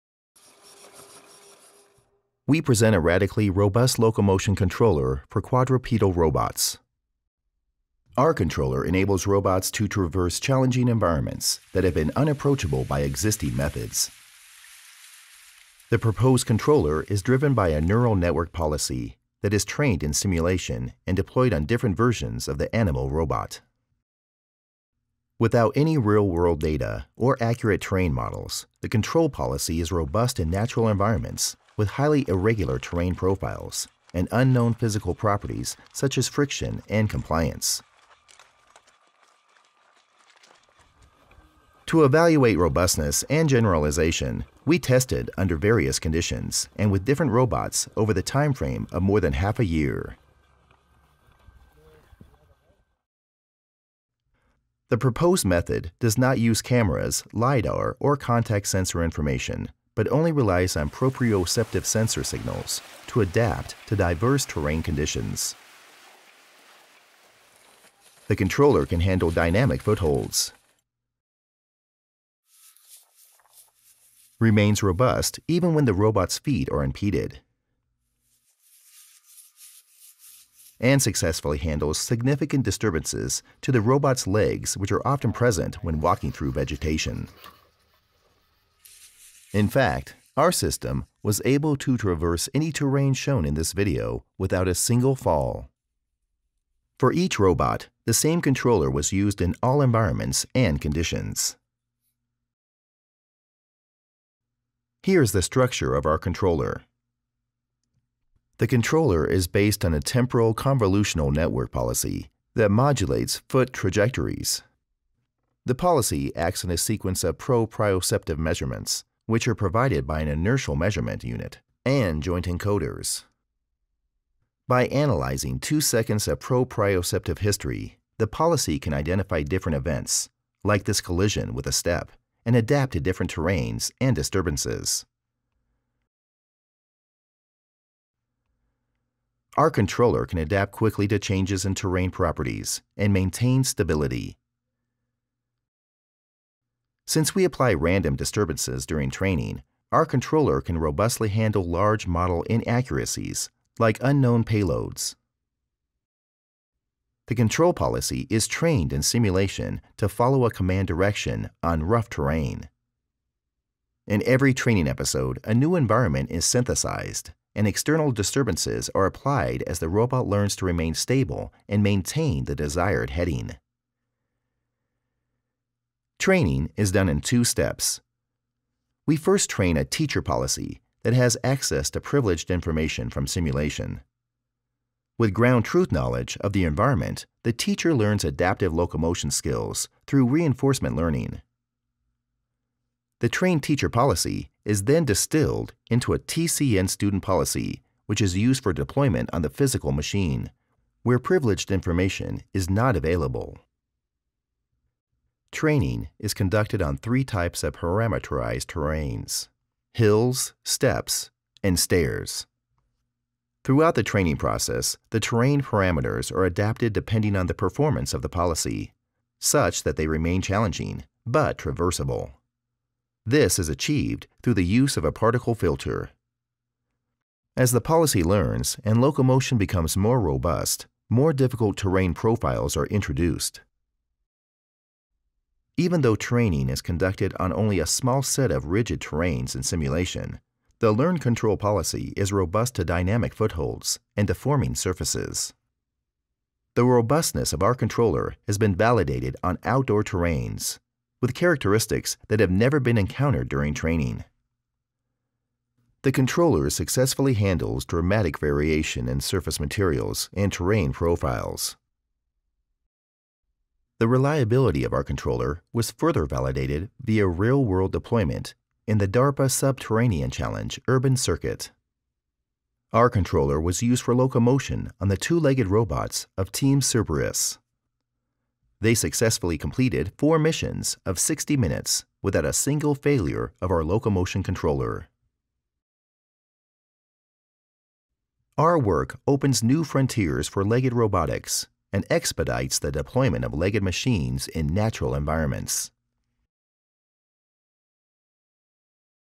Learning Quadrupedal Locomotion over Challenging Terrain
We present a radically robust locomotion controller for quadrupedal robots which is trained  using reinforcement learning in simulation. It uses no external sensing like cameras or Lidar and relies only on internal sensors like IMU and joint encoders.

Video by Joonho Lee